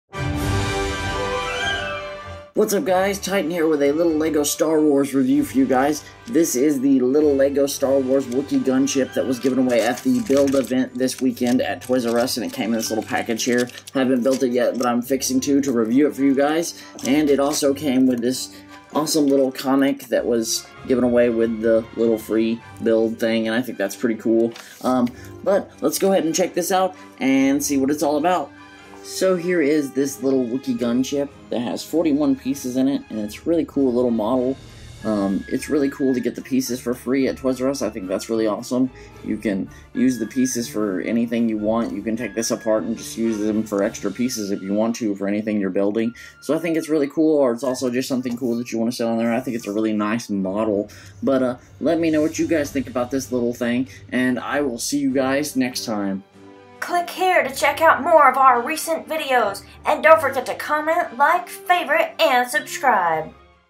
LEGO Star Wars Wookiee Gunship ToysRUs Build Event - BrickQueen
LEGO Wookiee Gunship Mini Build Review

Cool mini build from LEGO Star Wars at ToysRUs on Sunday May 3, 2015 from 12pm - 2pm. It's the Wookiee Gunship mini build!